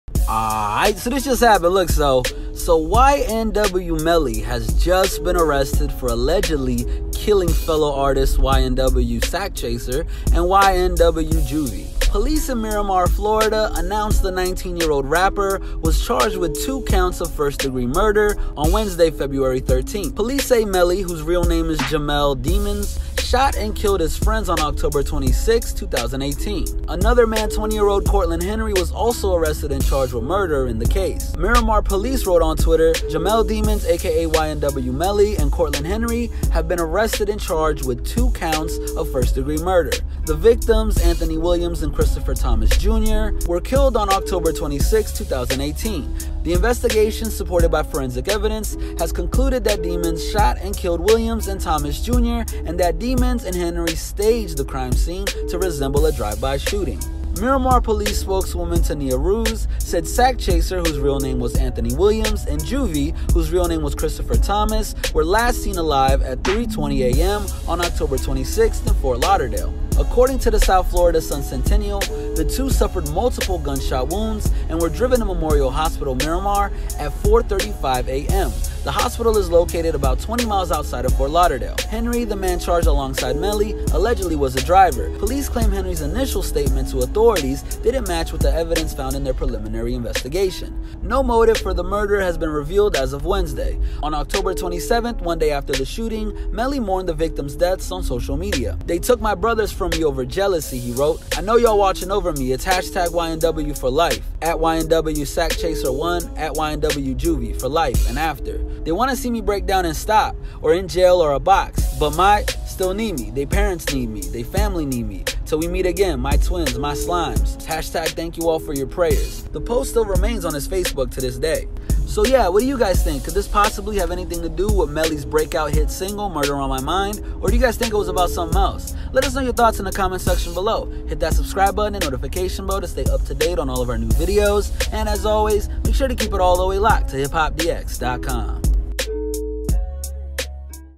YNW Melly Arrested For Assassinating His Own Friends
YNW Melly has been arrested for allegedly killing fellow artists YNW Sakchaser and YNW Juvy. Police in Miramar, Florida announced the 19-year-old rapper was charged with two counts of manslaughter on Wednesday (February 13).